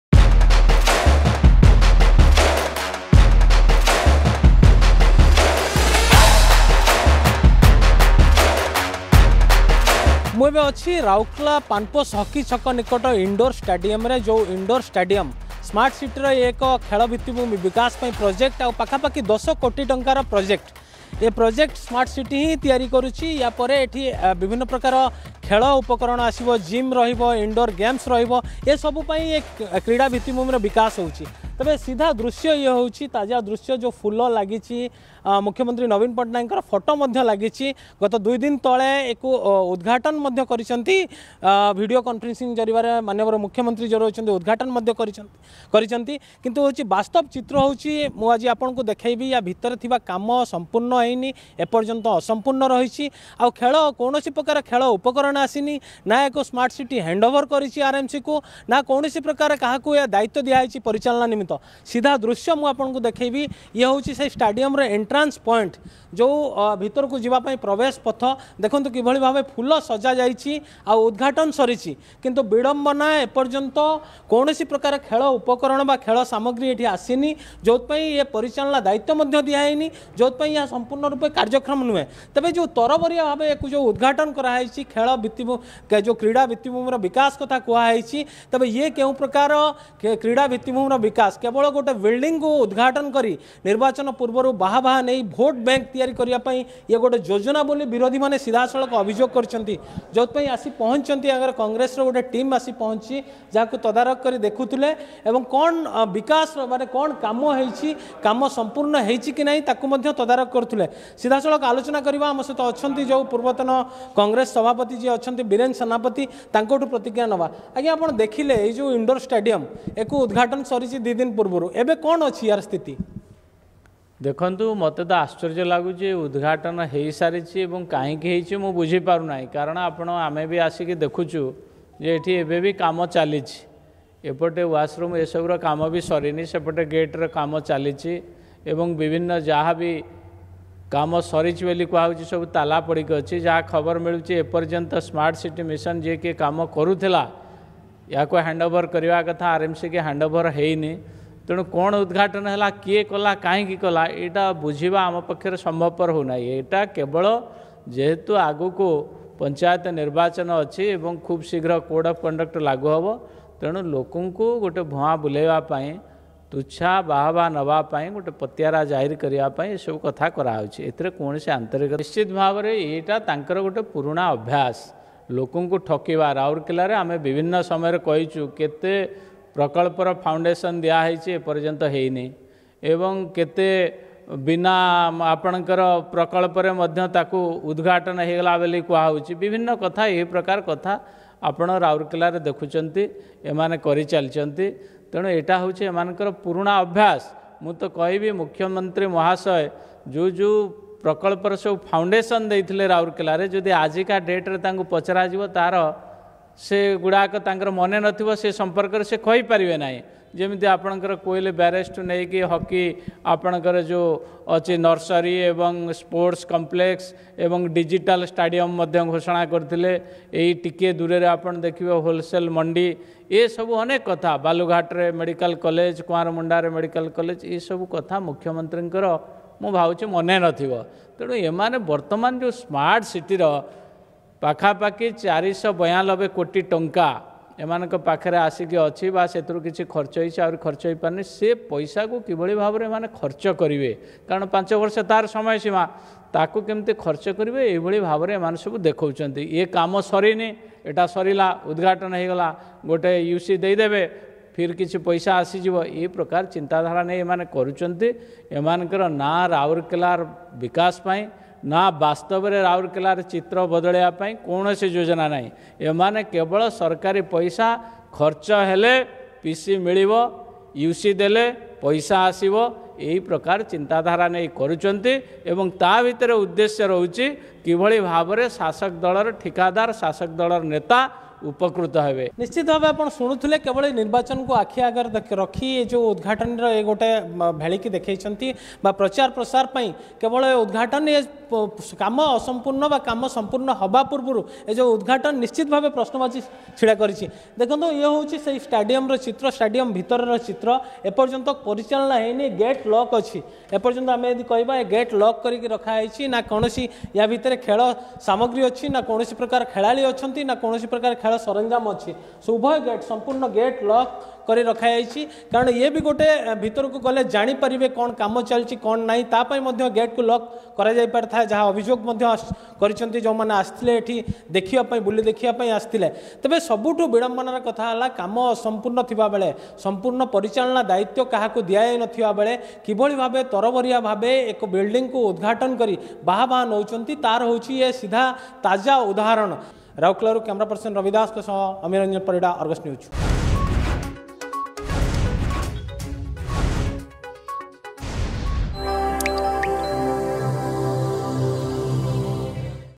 ଅଧପନ୍ତରିଆ ରାଉରକେଲା ପାନପୋଷର ଇଣ୍ଡୋର ଷ୍ଟାଡିଅମ
କାମ ସମ୍ପୂର୍ଣ୍ଣ ପୂର୍ବରୁ ତରବରିଆ ଭାବେ ରାଉରକେଲା ପାନପୋଷ ହକି ଛକ ନିକଟ ଇଣ୍ଡୋର ଷ୍ଟାଡିଅମ କୁ ଉଦ୍ଘାଟନ କଲେ ମୁଖ୍ୟମନ୍ତ୍ରୀ । ଏପର୍ଯ୍ୟନ୍ତ ଷ୍ଟାଡିଅମରେ କେବଳ ବିଲ୍ଡିଂ ନିର୍ମାଣ ସରିଛି କିନ୍ତୁ ଏପର୍ଯ୍ୟନ୍ତ ଆସିନୀ କୌଣସି ଖେଳ ସାମଗ୍ରୀ।